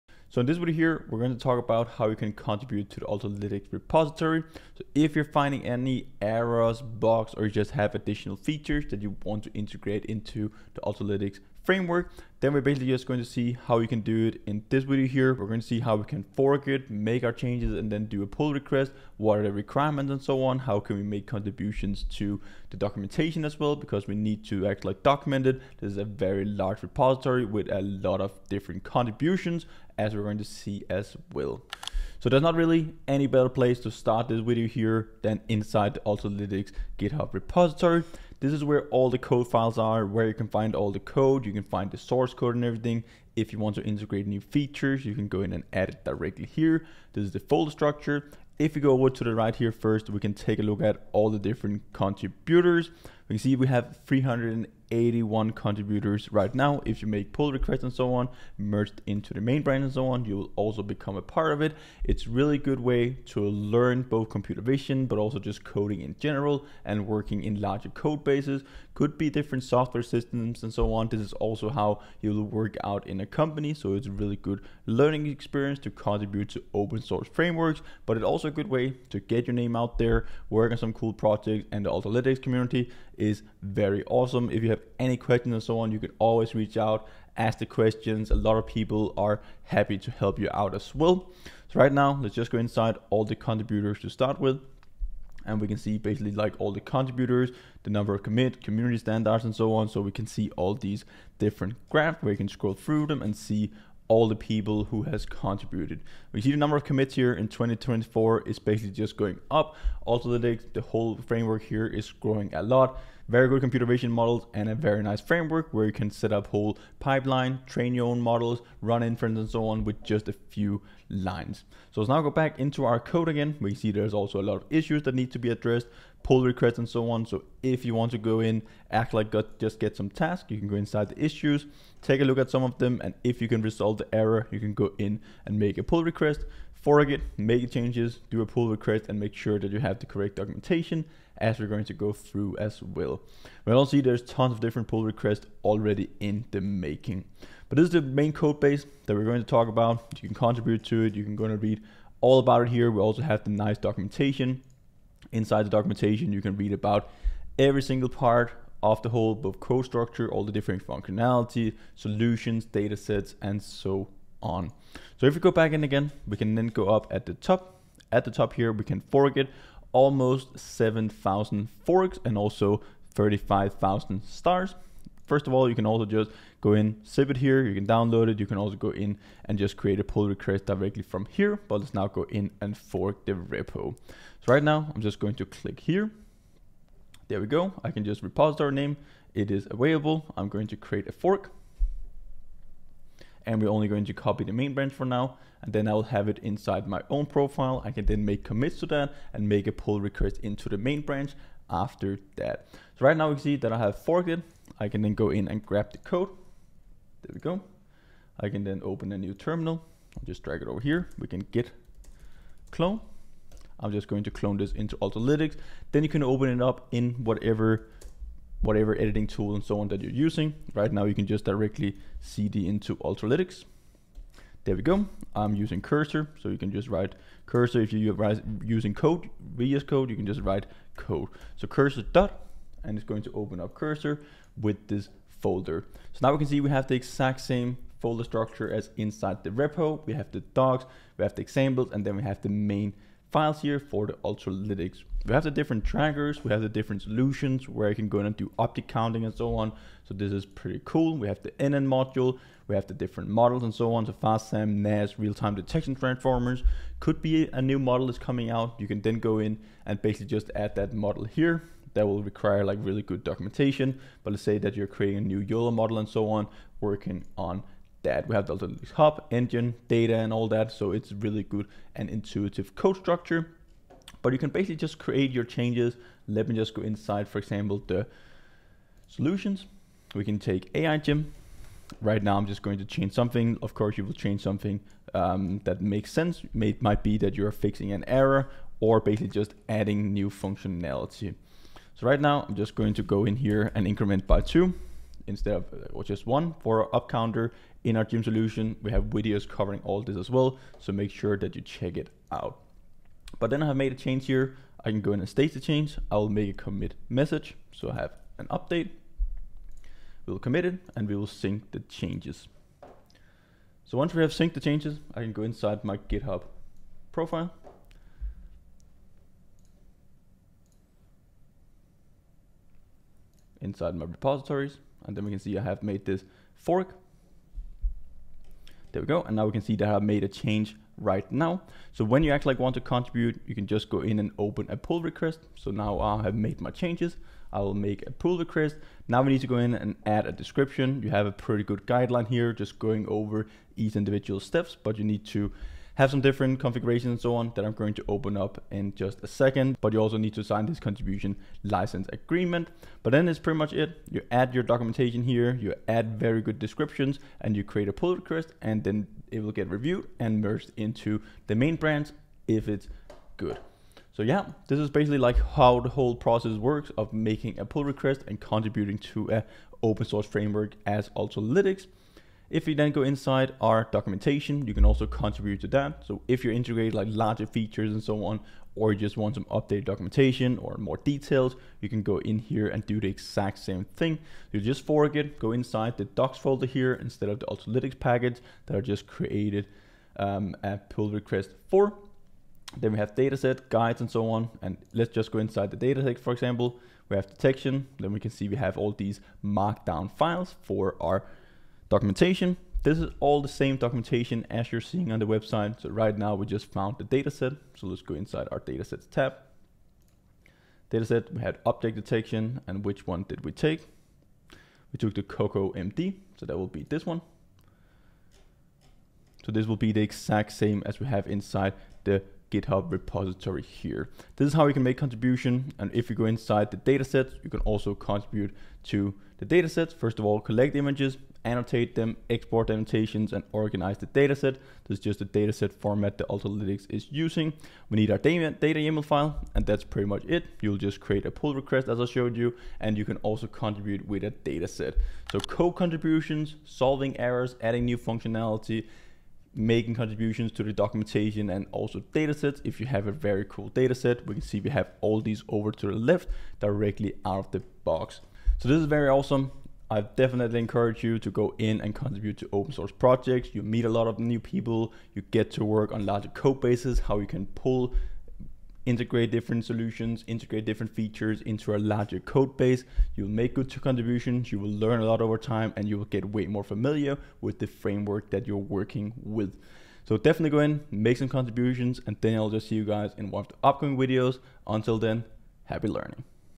How to Contribute to Ultralytics Repository | Ultralytics Models, Datasets and Documentation 🚀
In this tutorial, we will explore the process of contributing to the Ultralytics repository. This tutorial will walk you through the steps to fork, clone, and modify the Ultralytics codebase while ensuring seamless contributions to the project. Additionally, you will learn how to contribute to the Ultralytics documentation and datasets. This tutorial is created to help you get started with open-source contributions to Ultralytics.

Key Highlights:
00:00 - Introduction: Overview of the contribution process to the Ultralytics repository.
00:35 - GitHub Repository Overview: Quick tour of the Ultralytics GitHub repository.
02:44 - Forking the Repository: Step-by-step process to fork the Ultralytics repository.
03:24 - Cloning the Repository: Cloning the Ultralytics repository to your local machine.
03:36 - Codebase Changes: How to make changes in the existing Ultralytics codebase.
05:32 - Pull Request Workflow: How to open a Pull Request (PR) in the Ultralytics repository.
06:13 - Documentation Contributions: Steps to contribute to the Ultralytics documentation.
07:05 - Dataset Contributions: How to contribute to Ultralytics datasets effectively.
07:25 - Summary: Recap and next steps for contributing to Ultralytics.

Stay updated with our latest innovations in AI and computer vision.